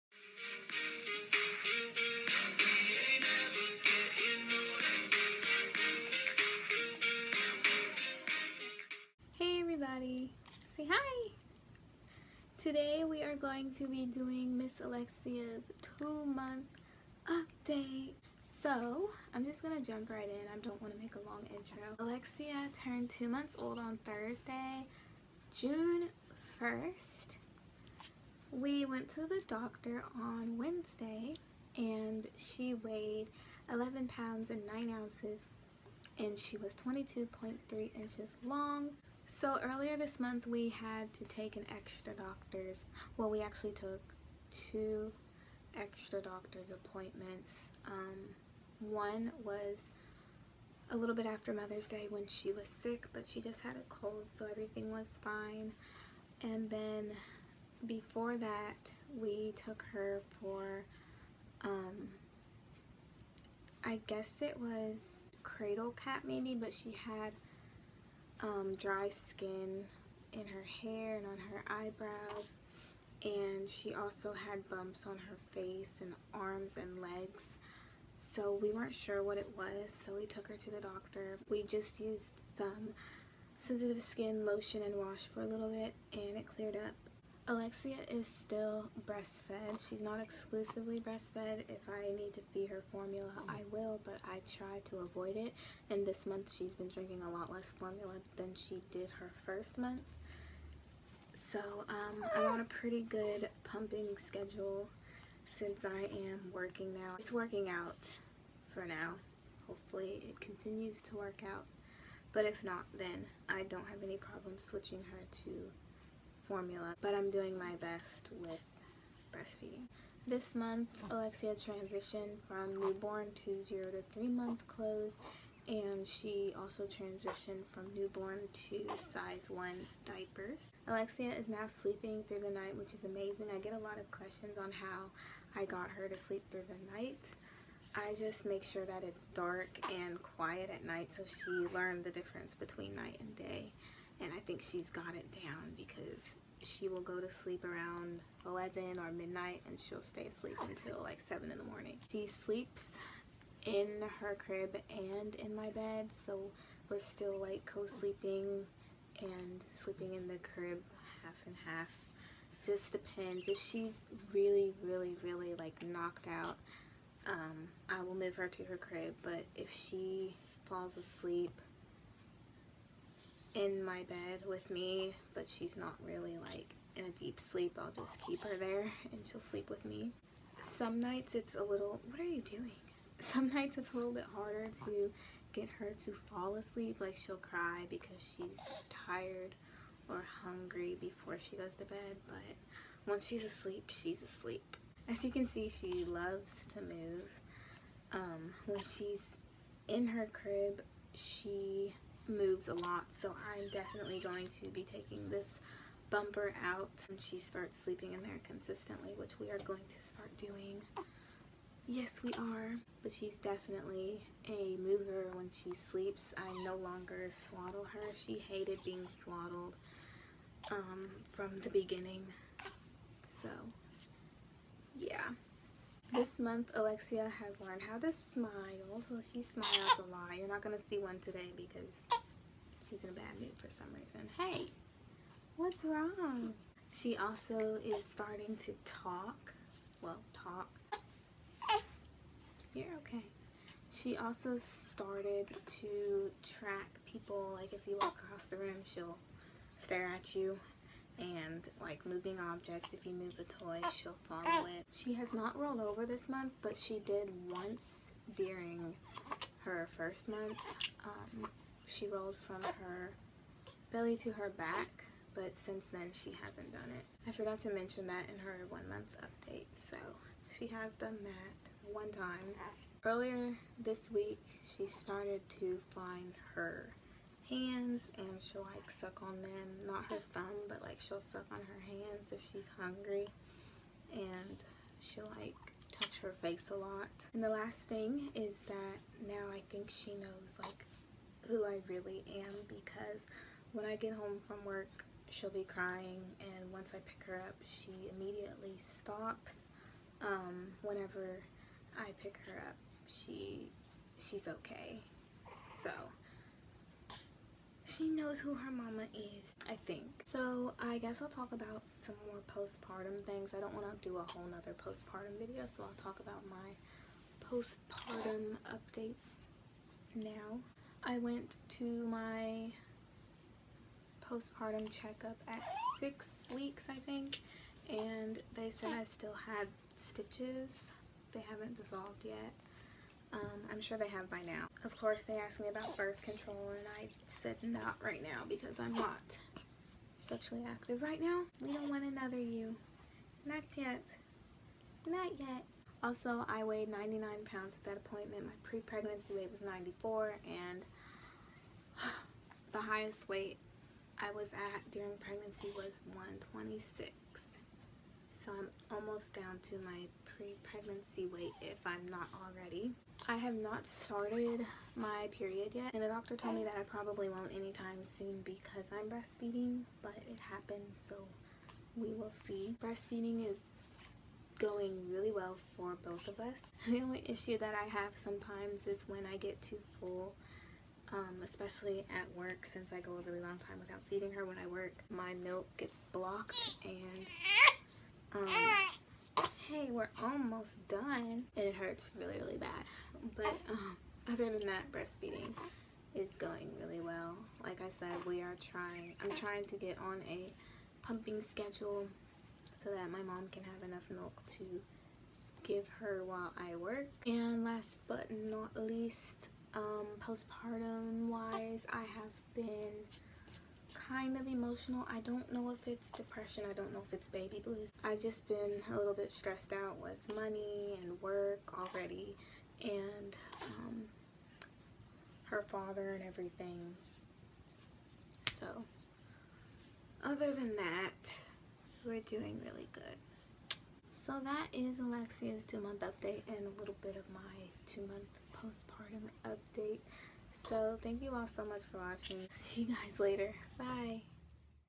Alexia's 2 Month Update!
Alexia turned 2 months old on June 1st! She is getting so big!

Leave video requests in the comments.